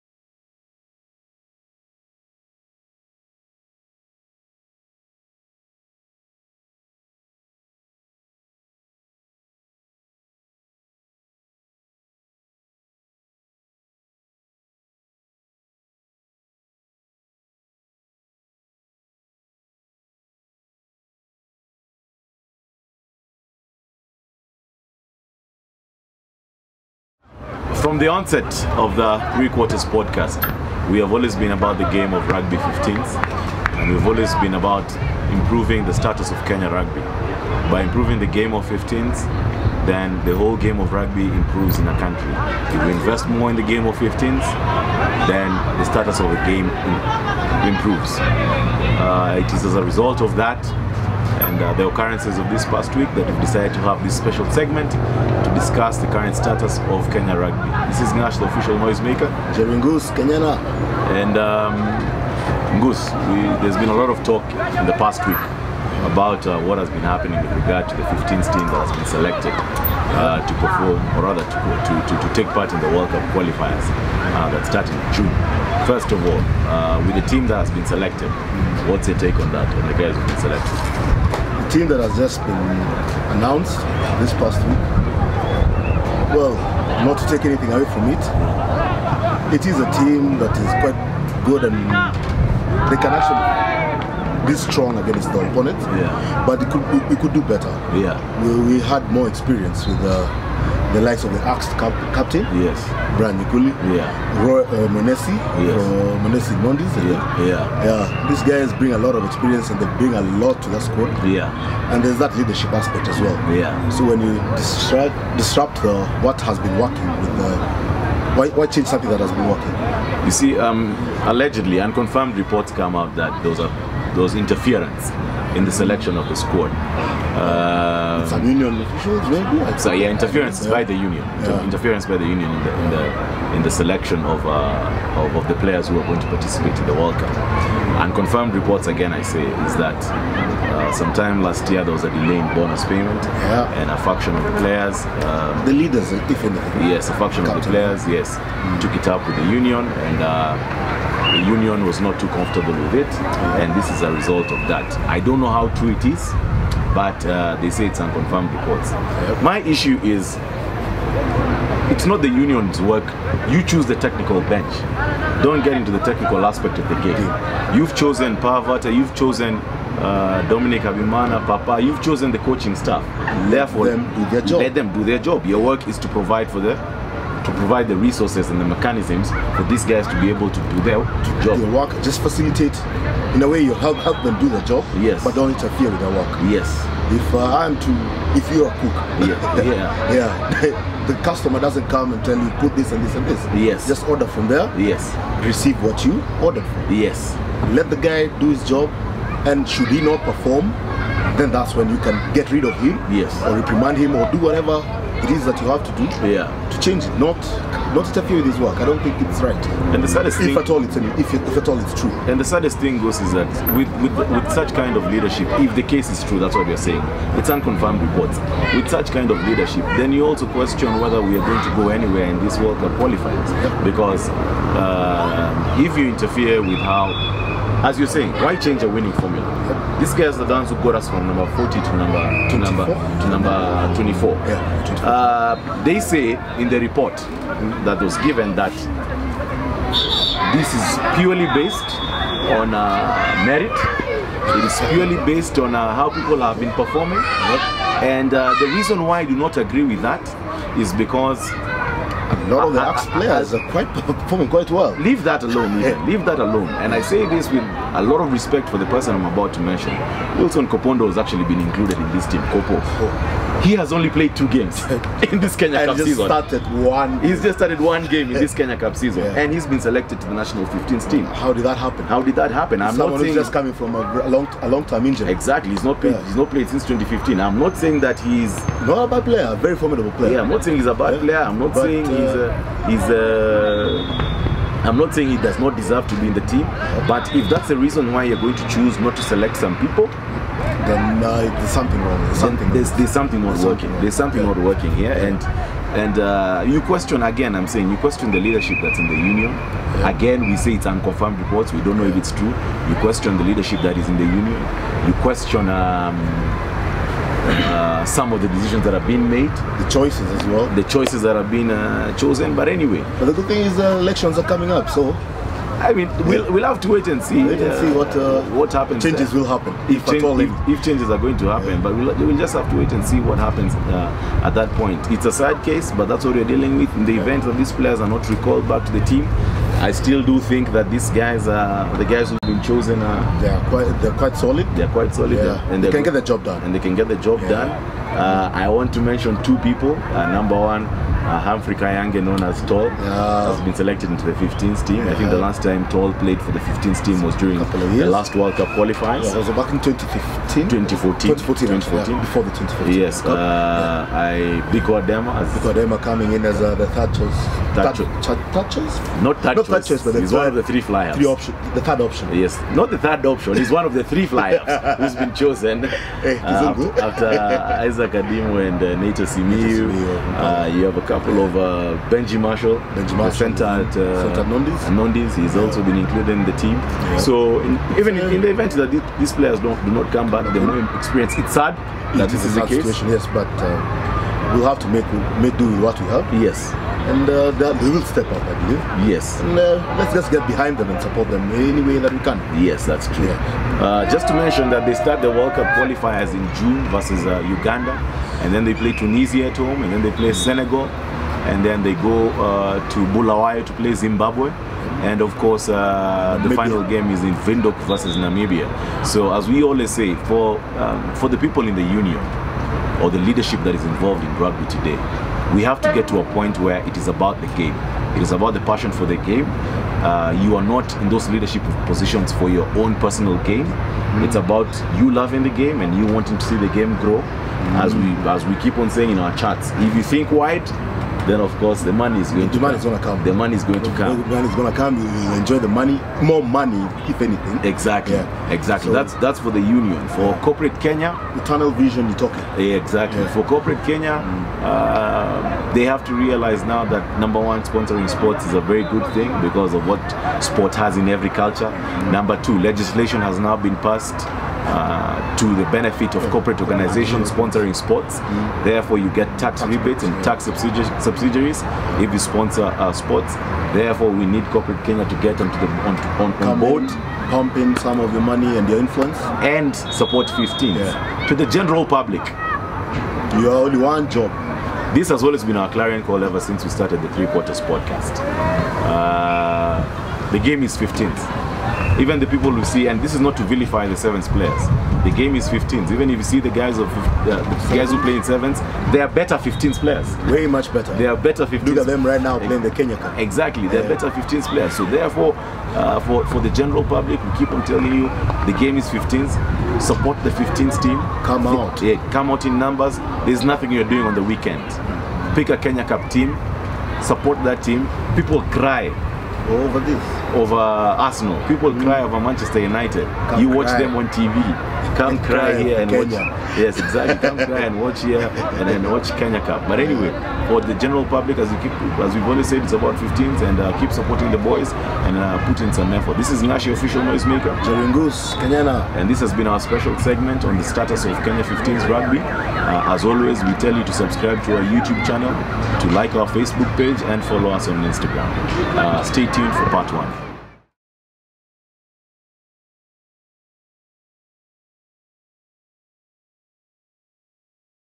Special Segment (Player Selection)
With the selection of the team that will be representing Kenya during the World Cup qualifiers came some oohs and aaahs on some high profile names missing from the list. The Three Quarters Podcast team sat down to talk about the team that has been chosen as well their thoughts on why some players must have missed the call up.